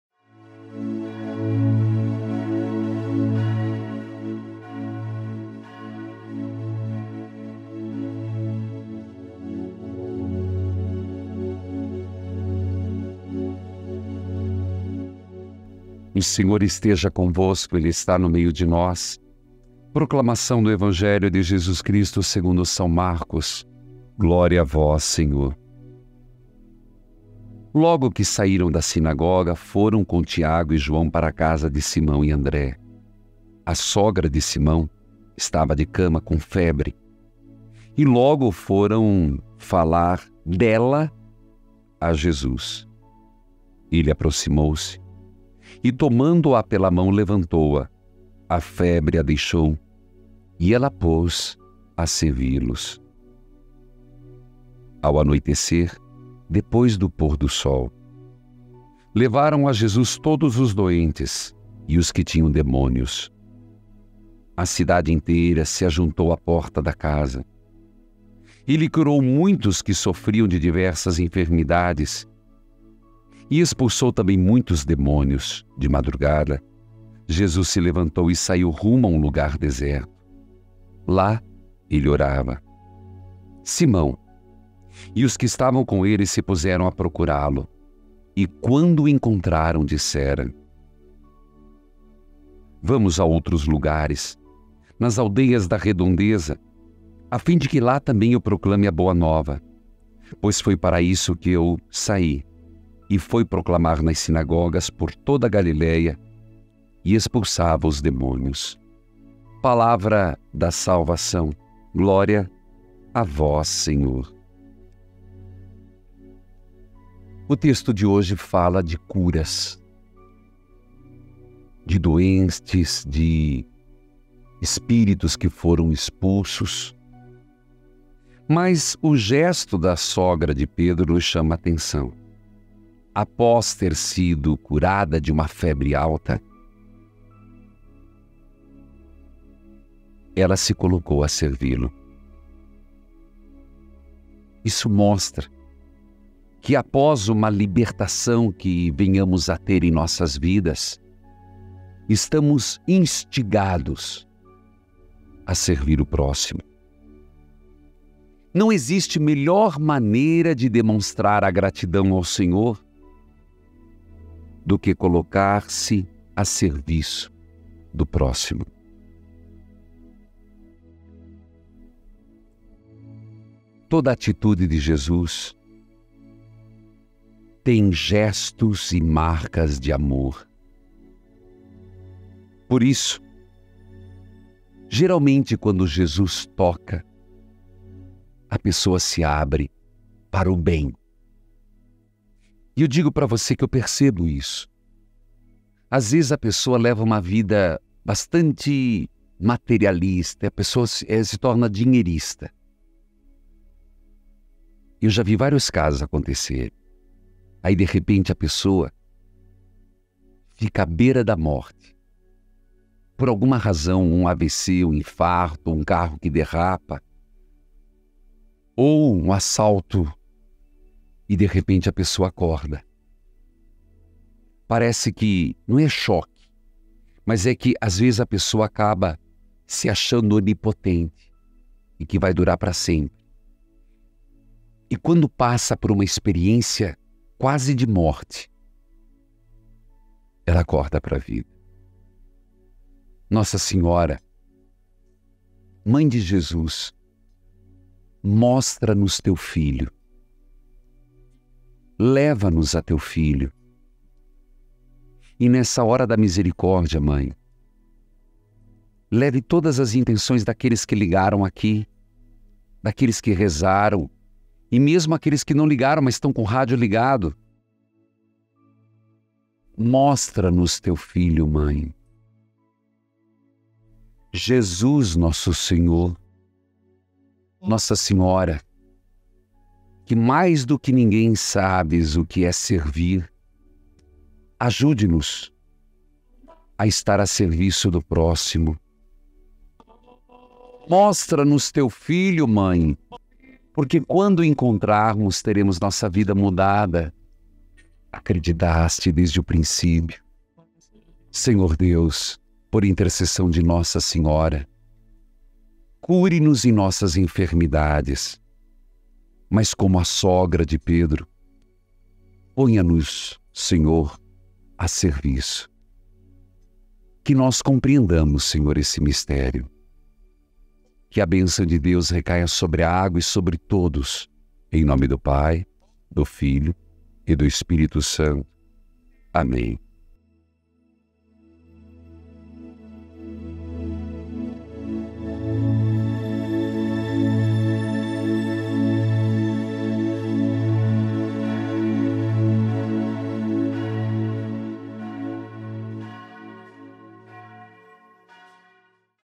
EVANGELHO DO DIA | 10/01/2024 | Mc 1,29-39
Acompanhe, diariamente, o Evangelho do Dia com Padre Reginaldo Manzotti. A Leitura Orante de hoje é o Evangelho de Jesus Cristo segundo Marcos  (Mc 1,29- 39).